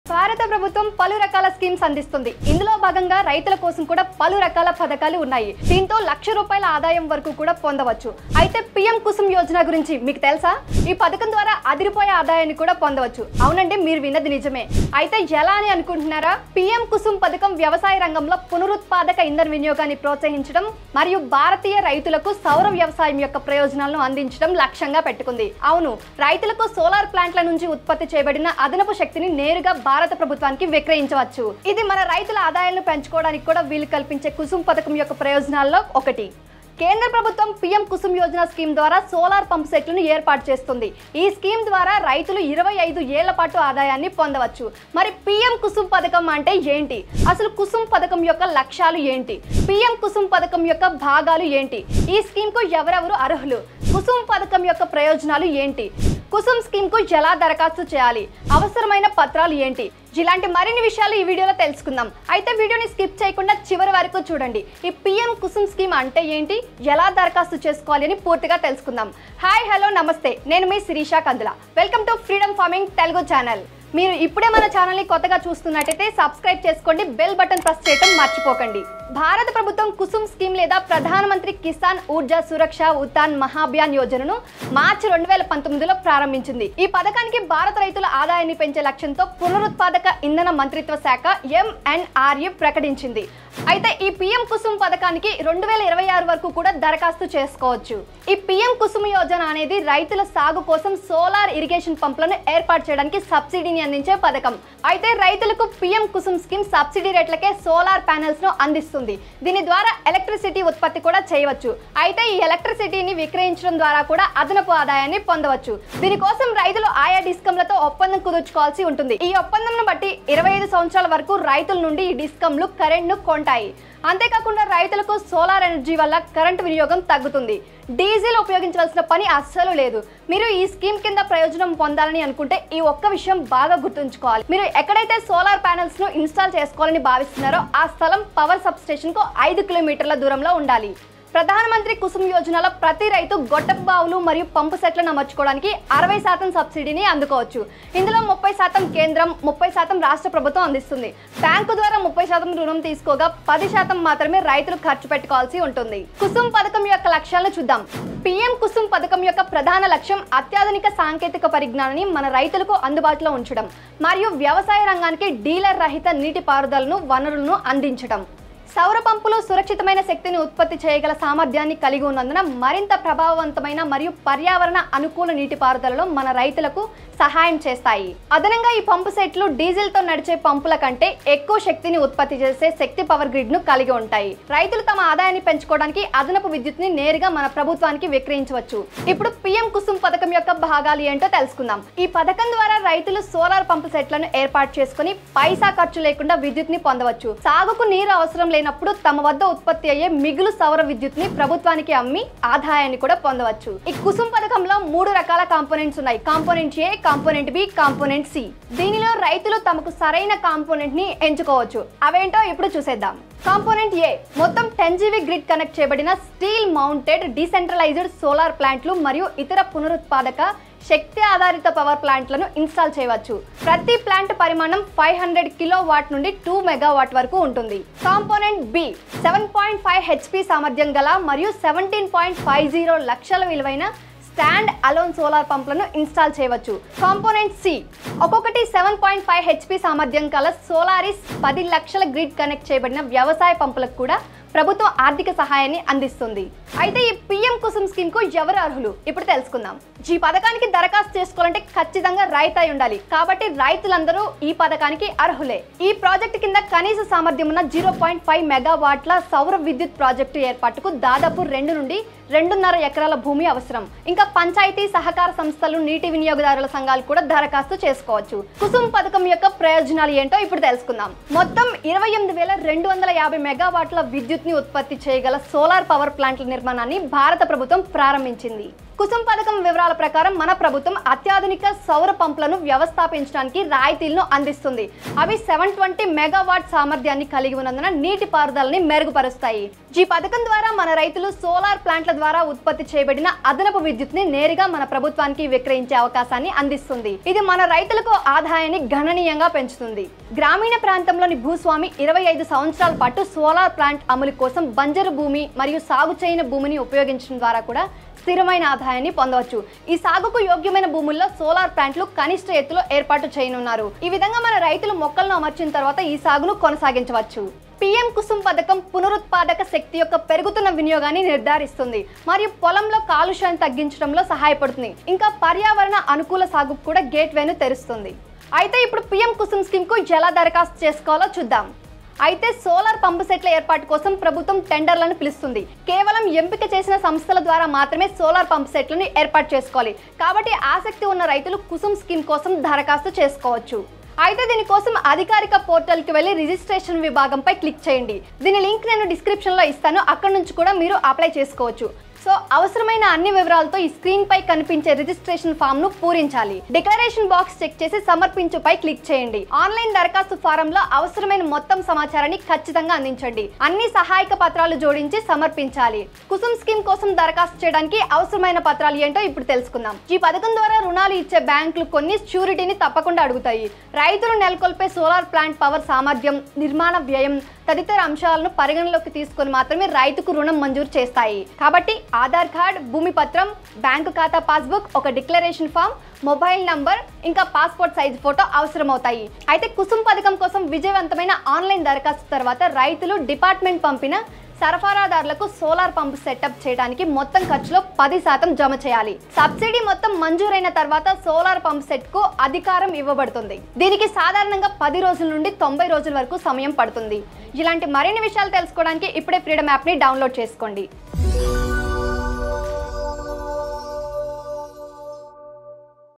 90% Subsidy from The Government | PM Kusum Yojana Details in Telugu | PM Kusum Apply
90% Subsidy from The Government | PM Kusum Yojana Details in Telugu | PM Kusum Apply